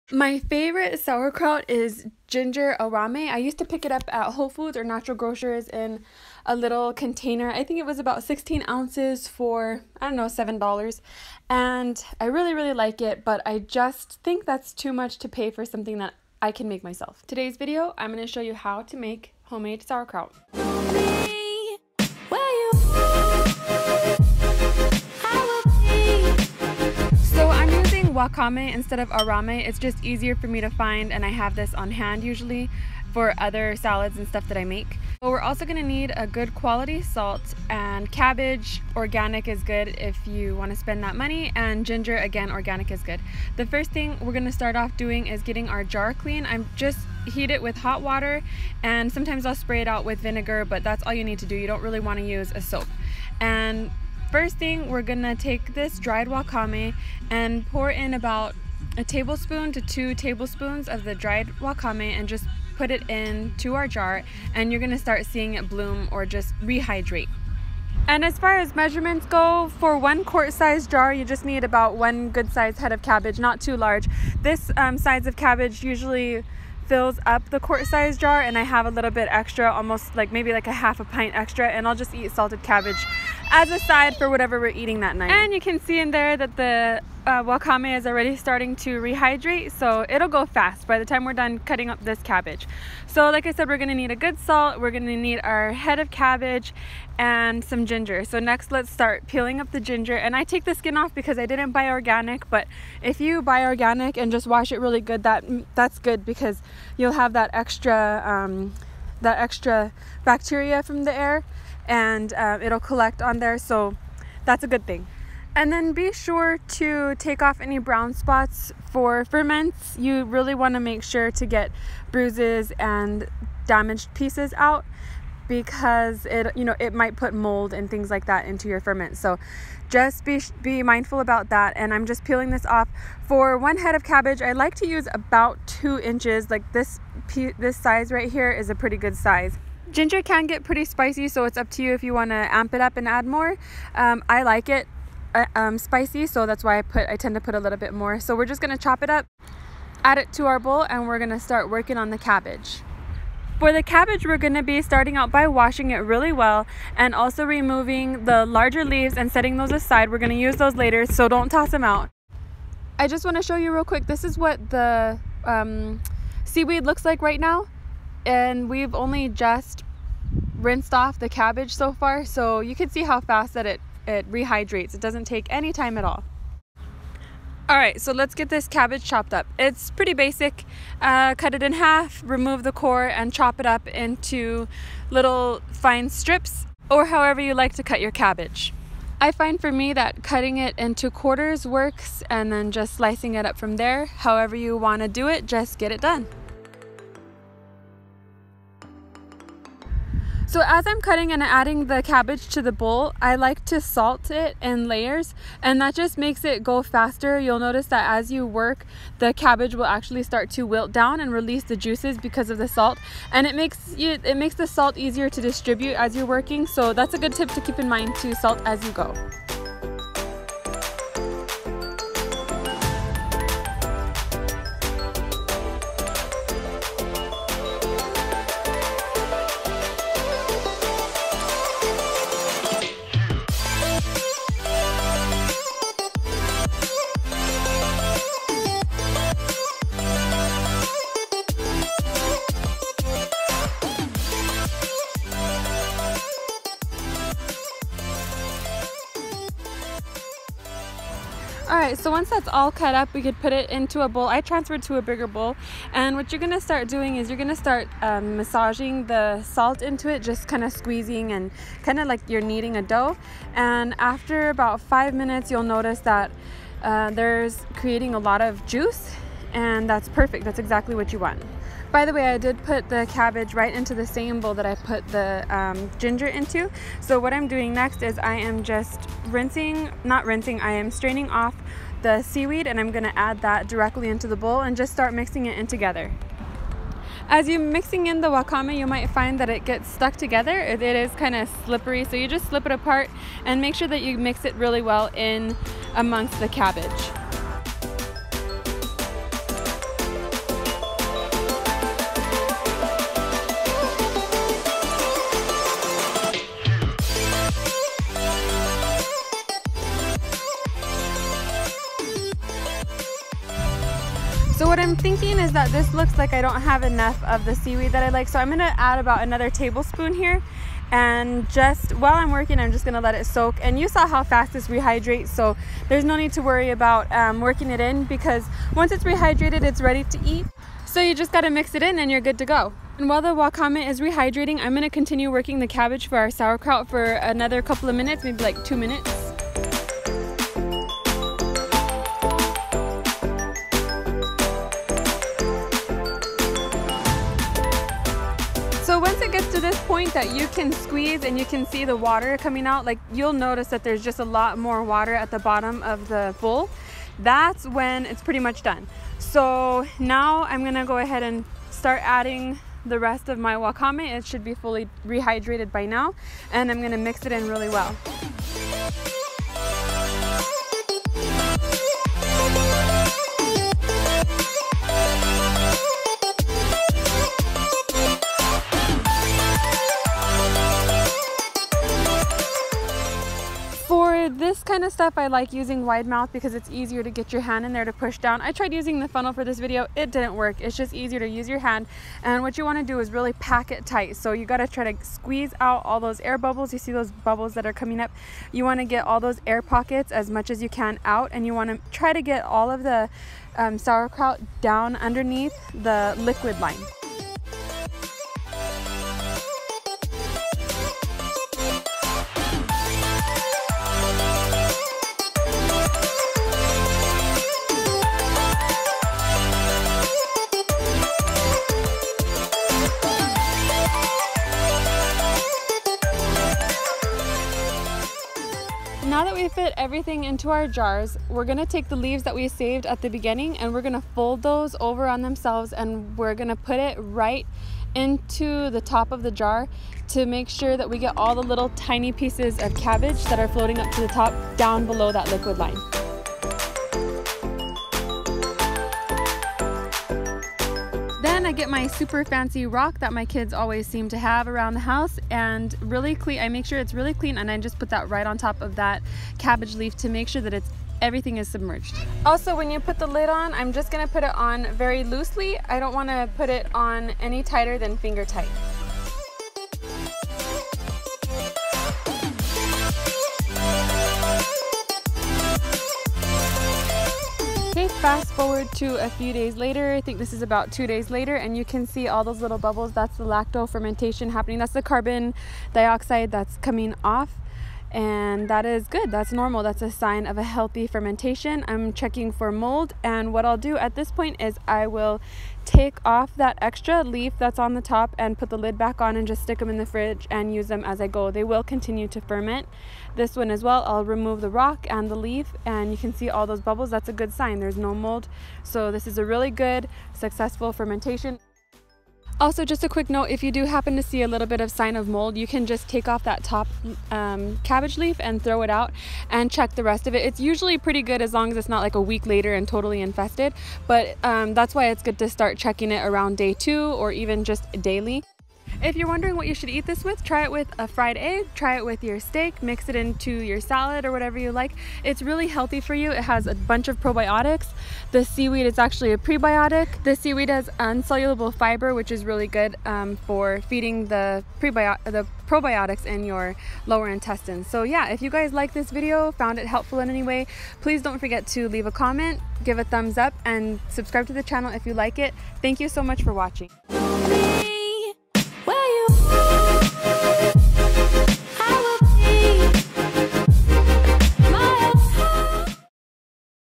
BOOST YOUR IMMUNITY W/LIVING FOODS! Homemade Ginger, Wakame Sauerkraut. BOOST IMMUNITY!
Did you know that 70% of our immune system lives in our guts? The bacteria that live in our small intestine is what defends of bodies against bad bacteria and virus. This sauerkraut is called a living food because of all of the good living bacteria in it. Keep your gut healthy & your immune system will be able to work and defend you more efficiently & effectively. This Easy Live food is full of probiotics and the wakame, a sea vegetable, has insoluble fiber which is a prebiotic. Let's just say this is a healthy food. If you have a habit of purchasing this living food from the health food store you know it costs a pretty penny. Sauerkraut is such an easy project to do at home. Give this recipe a try and let me know what you think!

For this recipe, I used wakame, here is a
Wide mouth quart jars
Fermentation weights, not needed IMHO but if you like to have all the gadgets

To read more about this benefits of seaweed, check out this article